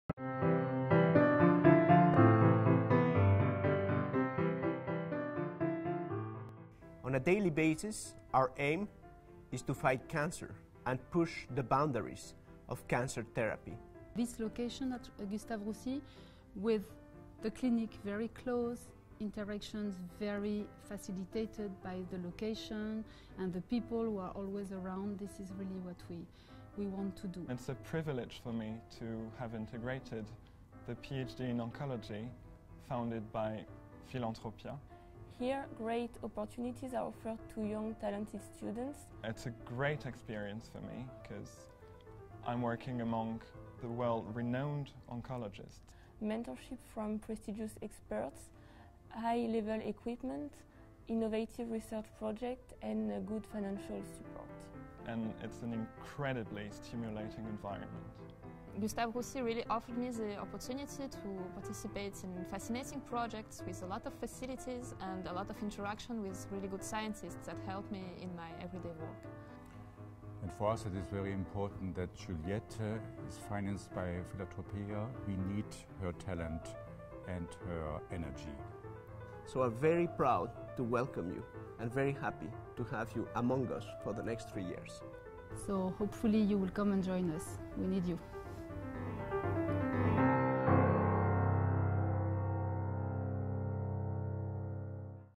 PhD in oncology: a great opportunity
Testimonies of Gustave Roussy's professors and PhD students "Course of Excellence in oncology – Fondation Philanthropia".